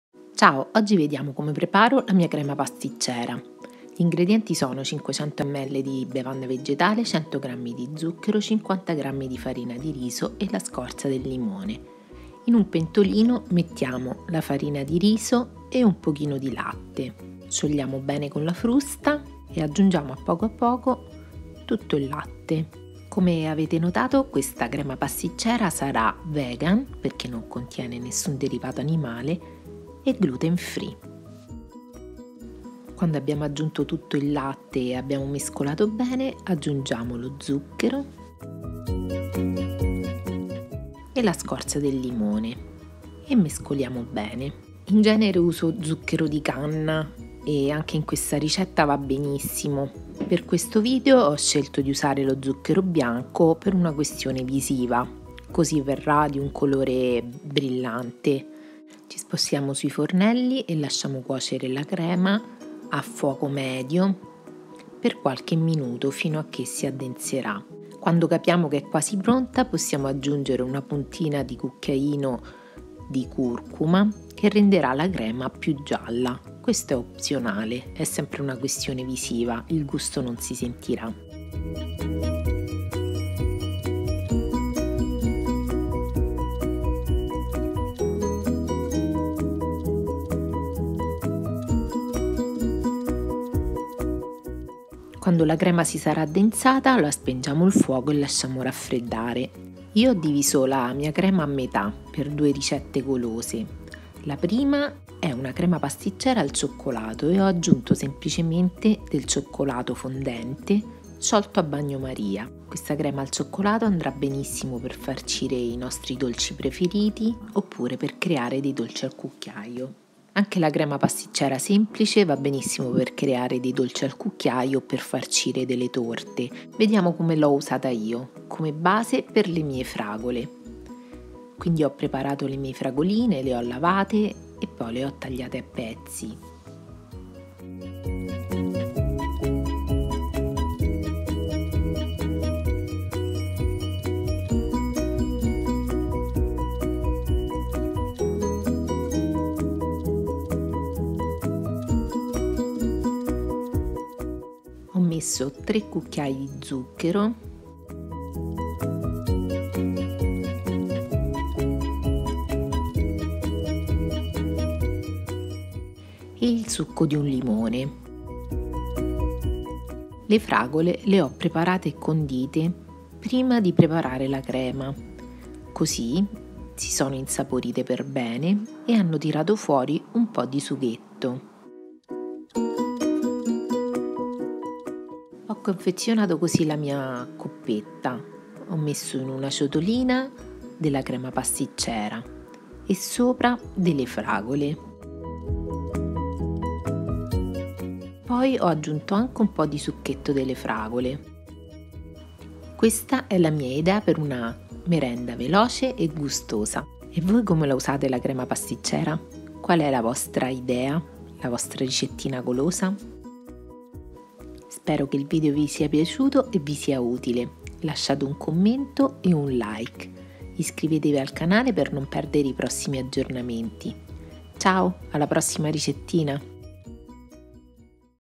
crema pasticcera - vegan - gluten free
Crema pasticcera Vegan? Gluten free? Sì è la ricetta del giorno

Lascia un 👍🏻 e un commento

A presto 🐝